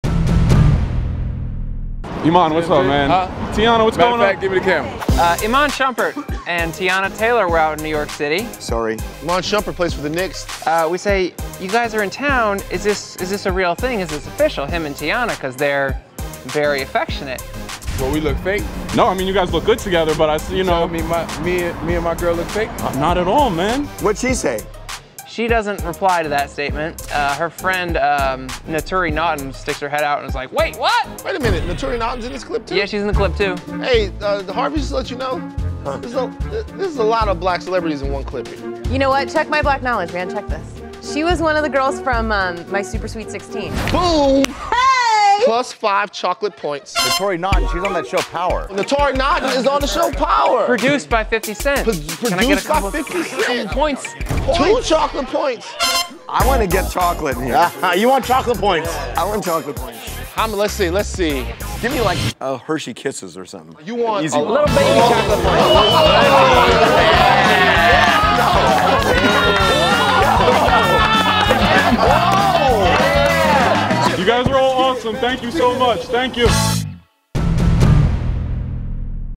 Iman Shumpert & Teyana Taylor: Real Deal? | TMZ
Is this true love, or set up for the cameras? We talk to New York Knicks star Iman Shumpert, and ask blankly, are he and Teyana dating?